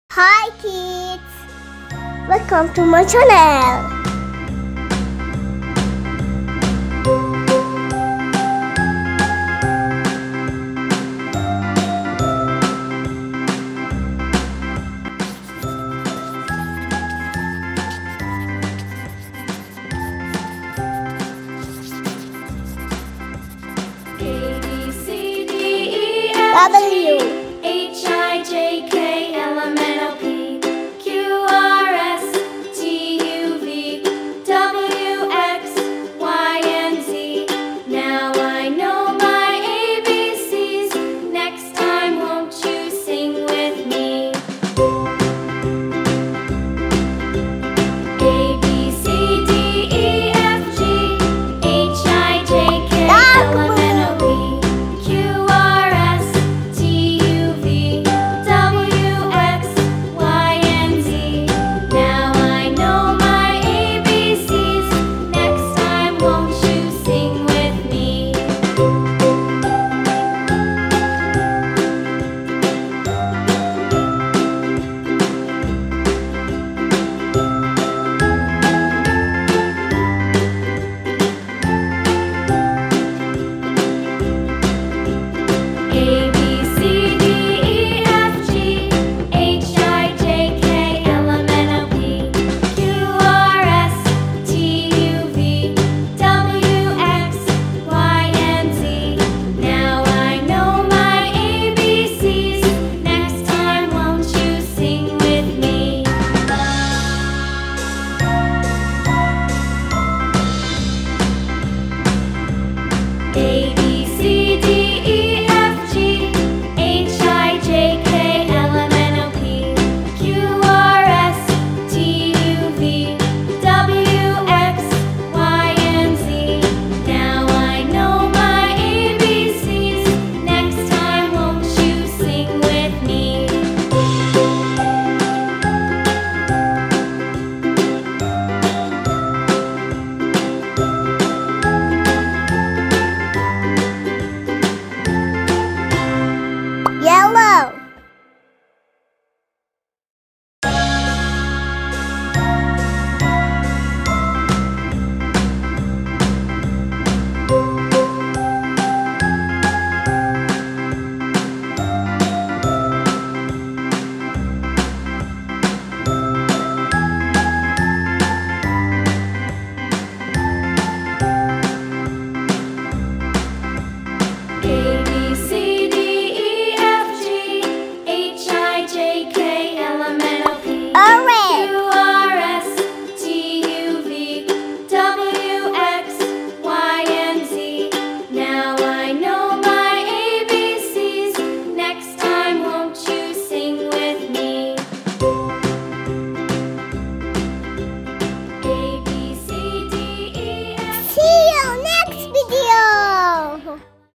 A,B,C, English alphabet Learn,Draw and Color
A,B,C, English alphabet Learn,Draw and Color for kids
How to Draw mandala alphabet Easy step by step for kids